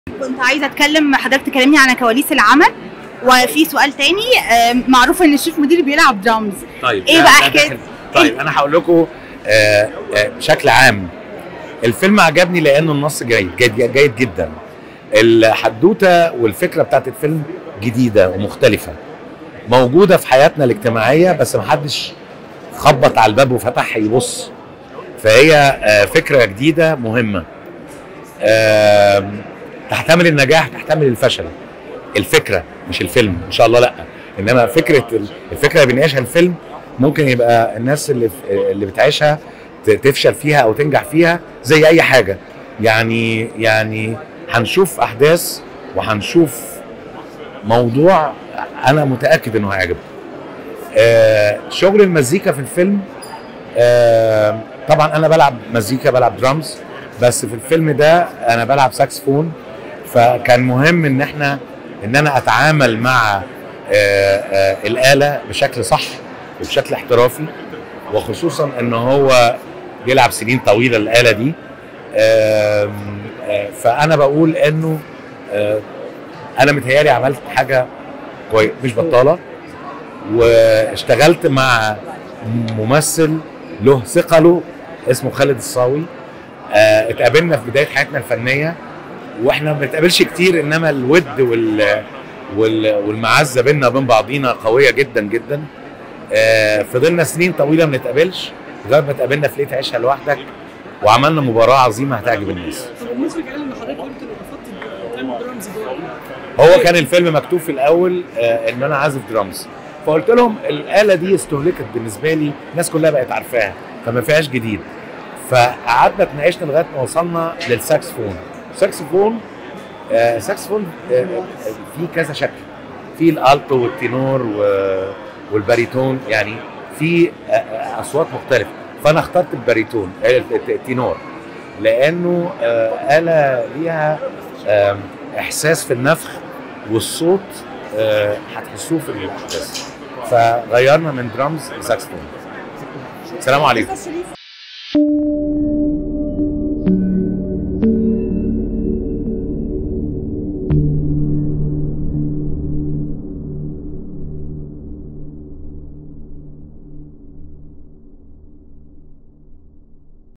شريف منير يكشف عن كواليس دوره فى ليه تعيشها لوحدك
ما عليك سوى الانتساب إلى هذه القناة للاستفادة من المزايا:
ElBaladOfficial#
ElBalad_Official#